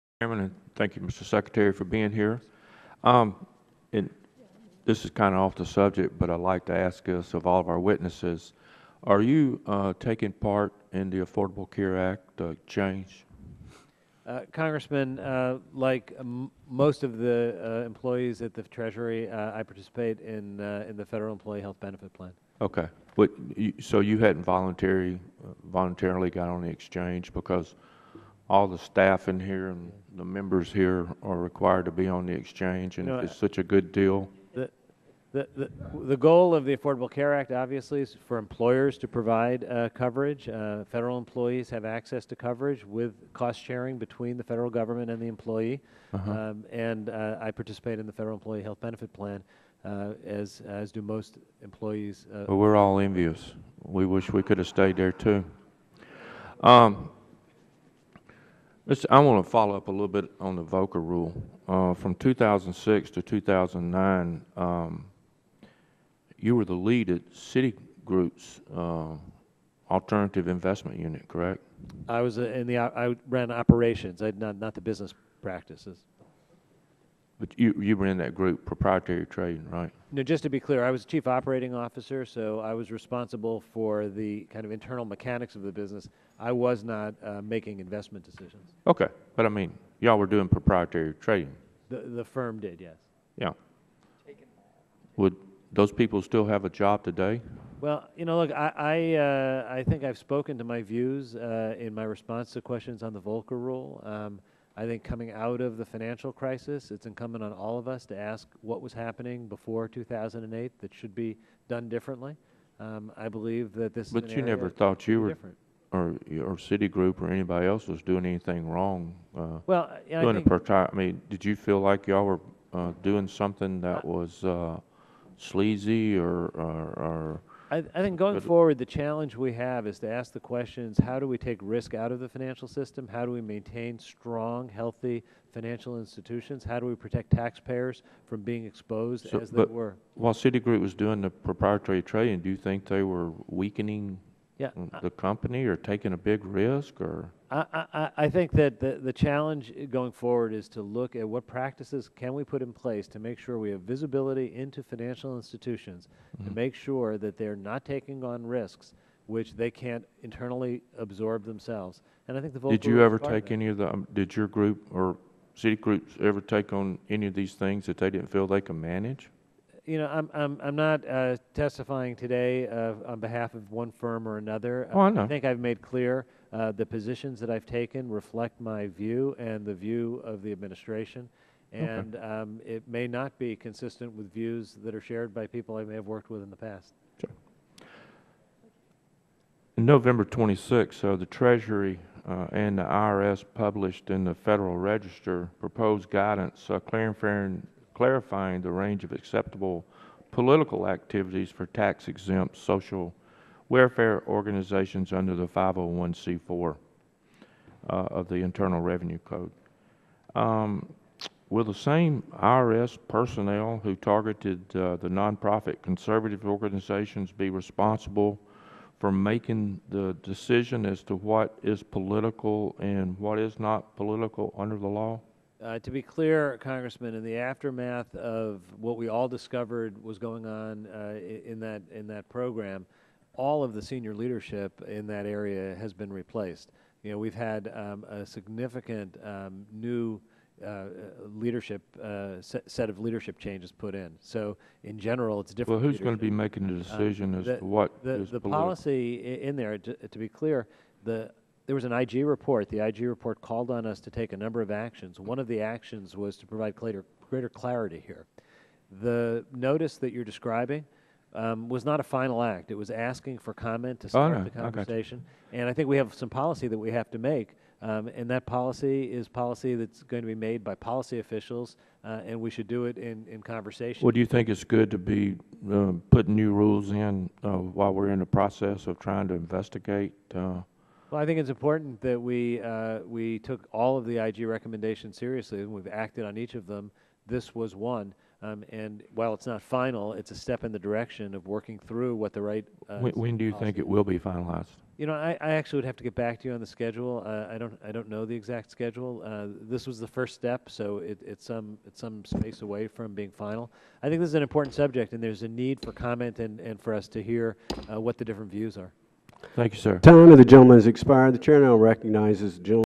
Westmoreland Questions Treasury Secretary Lew
On Thursday, December 12th, 2013, Congressman Lynn Westmoreland questioned U.S. Treasury Secretary John Lew in a House Financial Services Committee hearing entitled "Rethinking the Federal Reserve's Many Mandates on Its 100-Year Anniversary." Rep. Westmoreland opens the questioning with asking Secretary Lew if he is taking part in ObamaCare, where Sec. Lew said he is not.